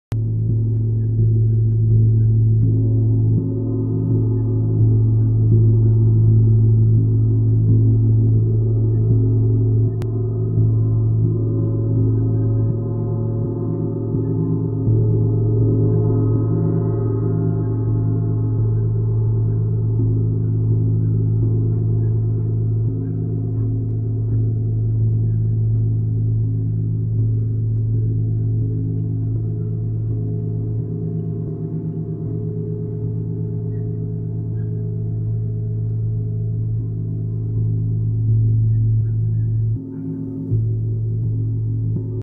GonGaia x Vani Levi
Meditacion con Gong en Monumento megalitico en Santa Catarina.